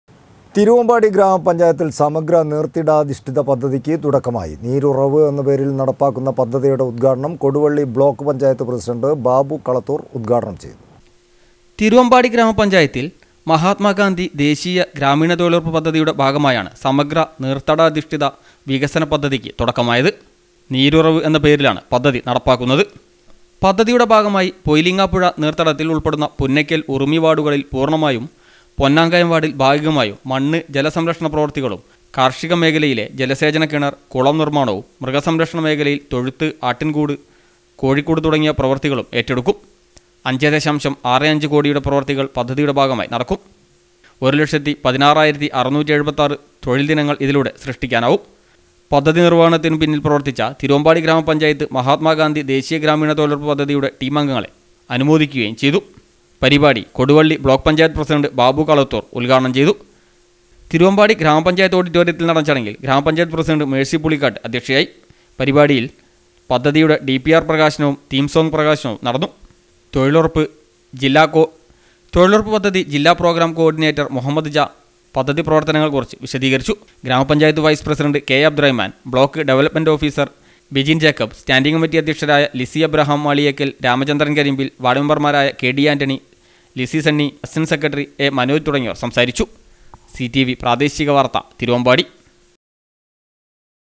തിരുവമ്പാടി ഗ്രാമപഞ്ചായത്തിൽ സമഗ്ര തീർത്തടാധിഷ്ഠിത പദ്ധതിക്ക് തുടക്കമായി
തിരുവമ്പാടി ഗ്രാമപഞ്ചായത്തിൽ സമഗ്ര തീർത്തടാധിഷ്ഠിത പദ്ധതിക്ക് തുടക്കമായി. നീരുറവ് എന്ന പേരിൽ നടപ്പാക്കുന്ന പദ്ധതിയുടെ ഉദ്ഘാടനം കൊടുവള്ളി ബ്ലോക്ക് പഞ്ചായത്ത് പ്രസിഡൻ്റ് ബാബു കളത്തൂർ ഉദ്ഘാടനം ചെയ്തു